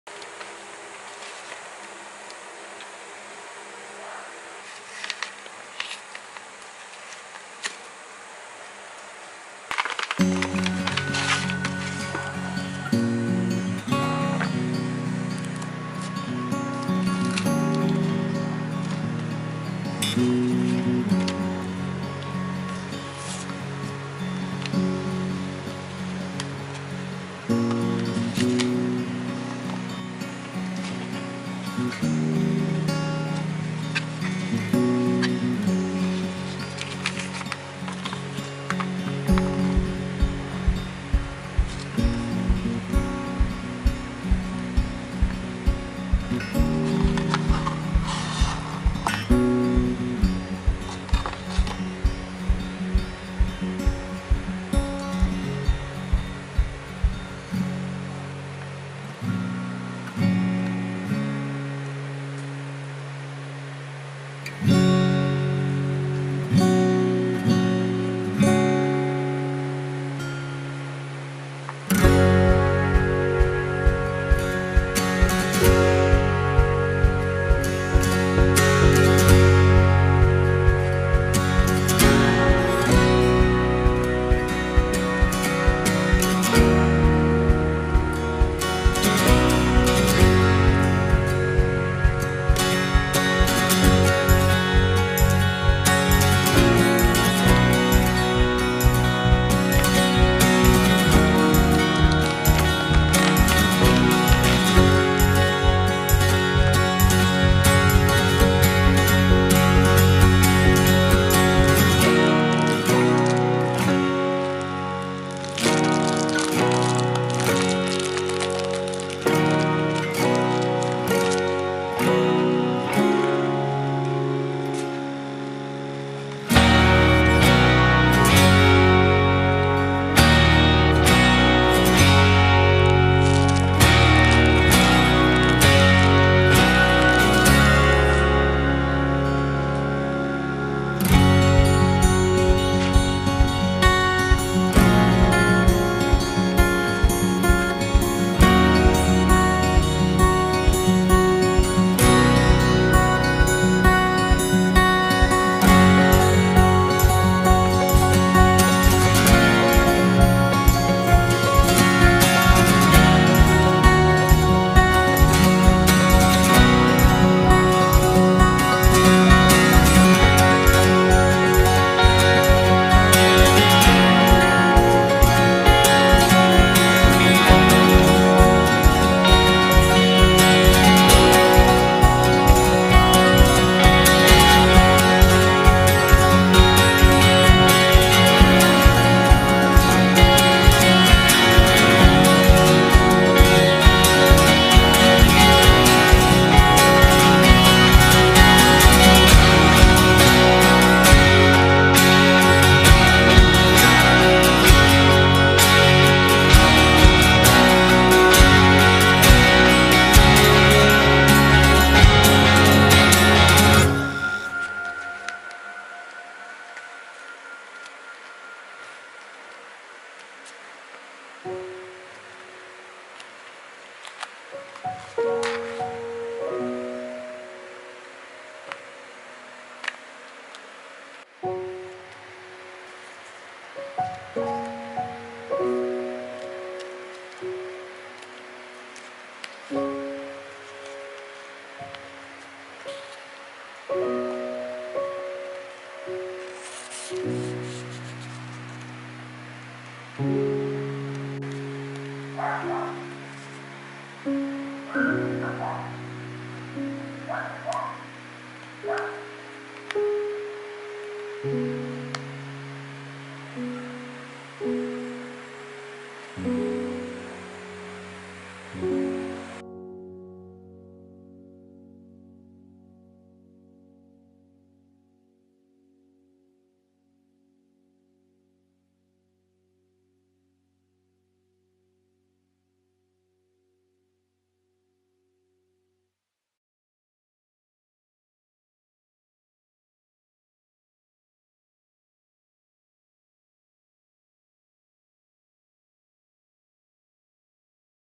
— my new traveler's notebook passport in camel | unboxing and set up
i fell so in love with my regular traveler's notebook that i wanted to get another one in passport size. this size is so much fun and so cute and so handy! i am using mine as a wallet too. he is my newest baby!

i got everything from Scribe Philippines